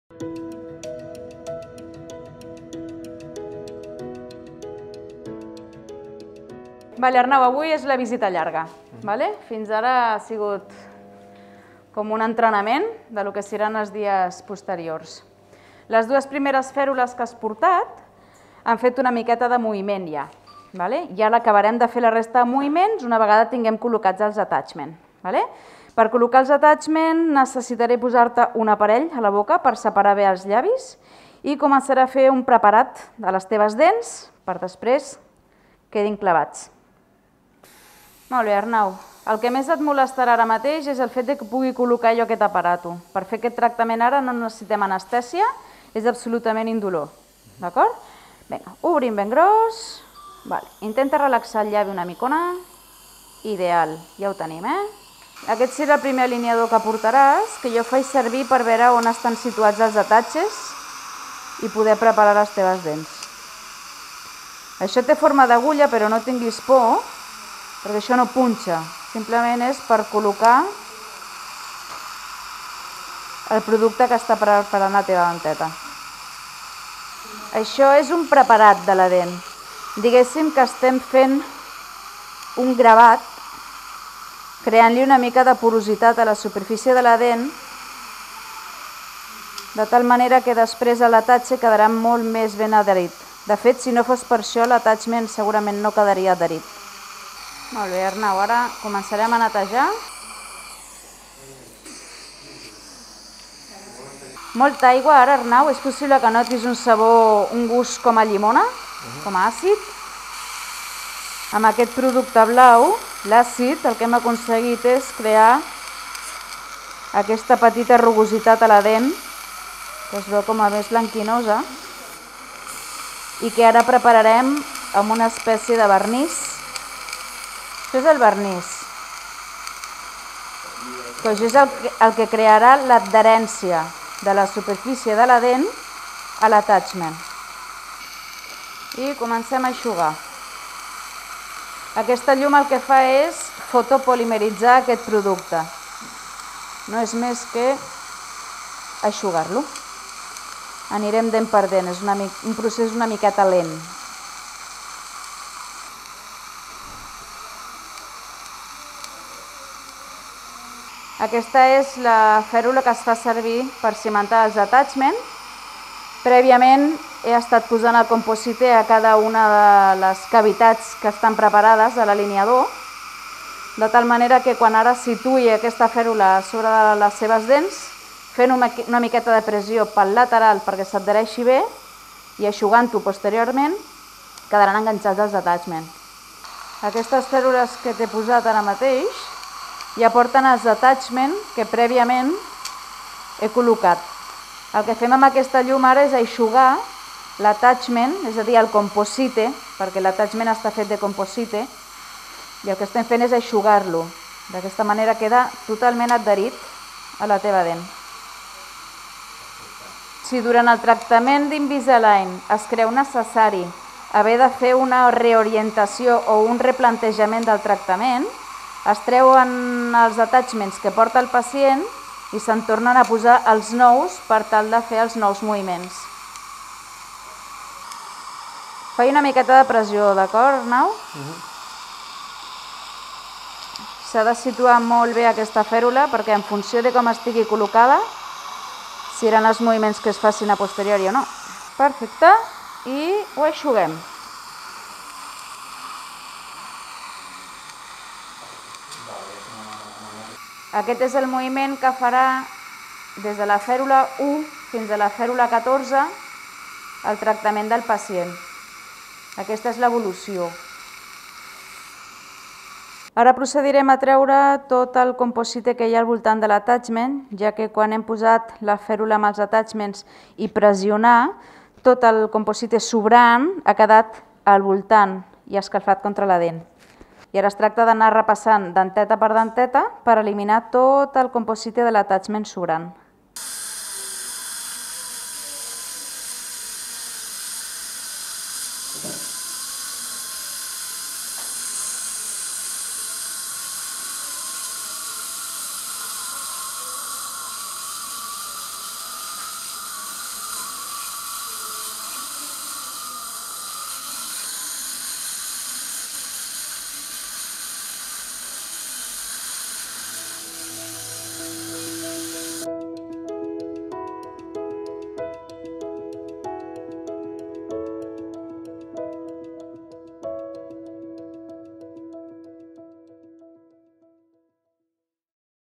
Invisalign | attachements | 2ª visita Arnau
Hoy vivimos la segunda visita de Arnau , en su tratamiento de Invisalign; esta es la visita más larga o duradera ya que hemos de colocar los attachements.